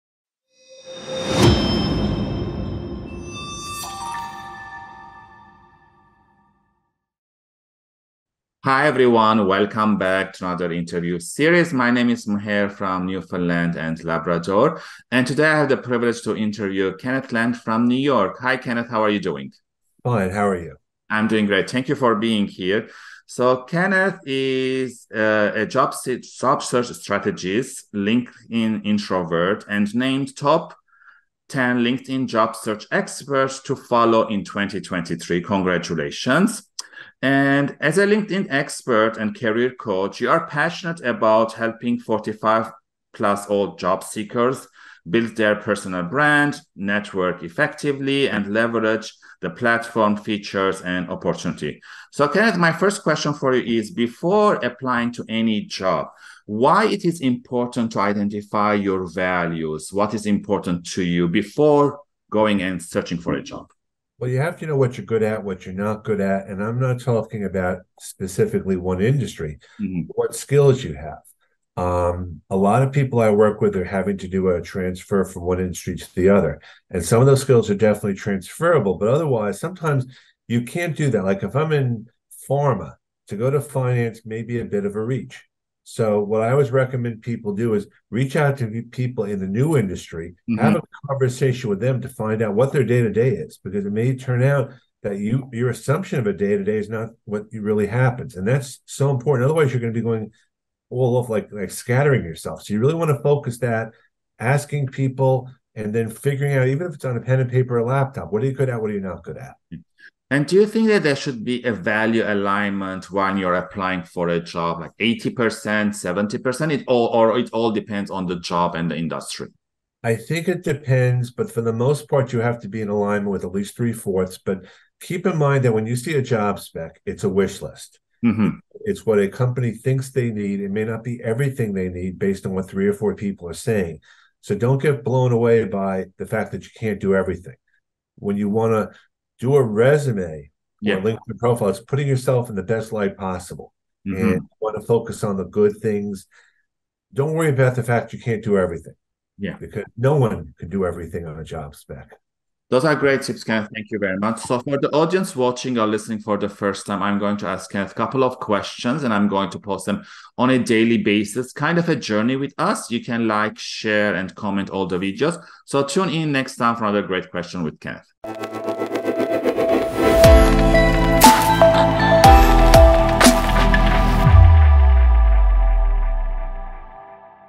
Why IDENTIFYING YOUR VALUES are important before applying to a JOB?
As a jobseeker it is important to identify your personalvalues and your strenghts, before applying to any

WHY?

According to Kenneth Lang, you need to know what you good at and what you are not good at

It is important to identify

Is it important to have value alignment with the company you are applying? (watch the video)